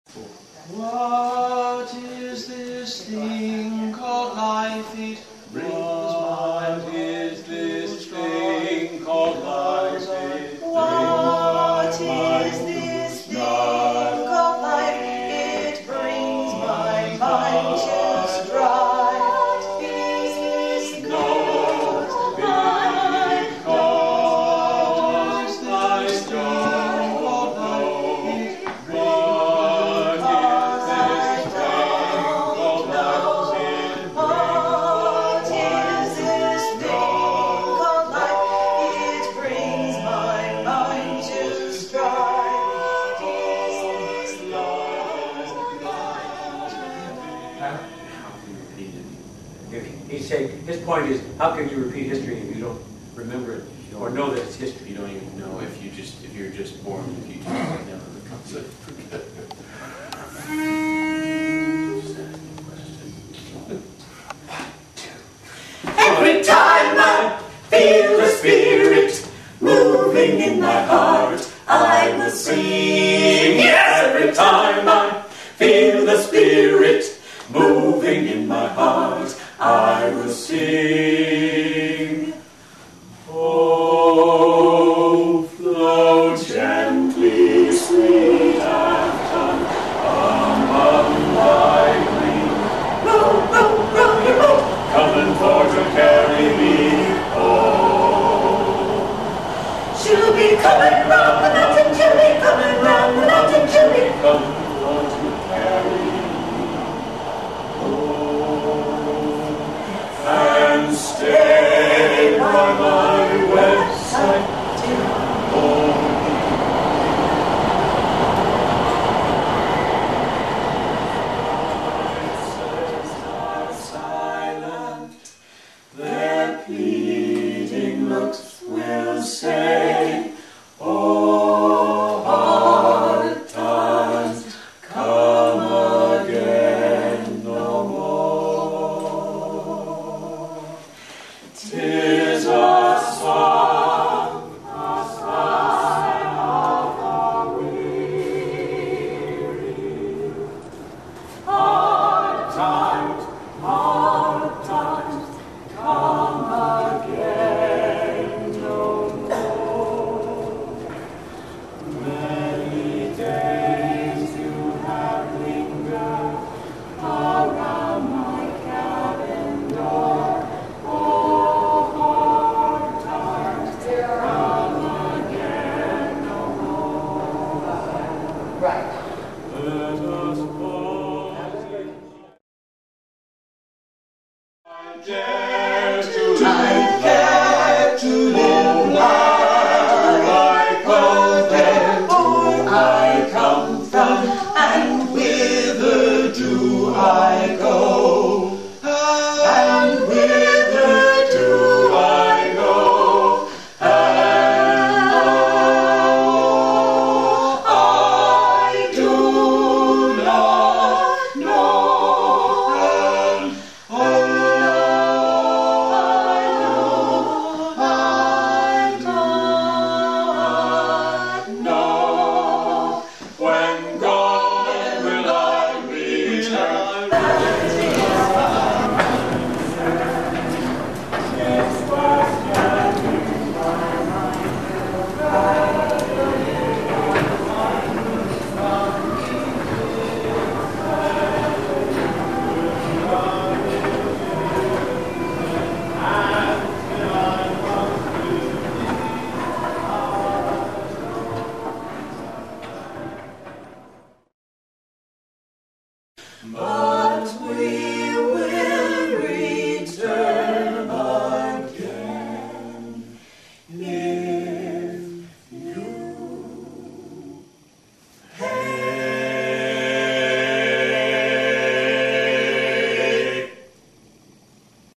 The Moorpark Master Corral
This is a group of professional actors/singers I have been working with
to create unique and unusual musical experiences for varied
audiences... Garage door openings... yard sales and expensive cruises!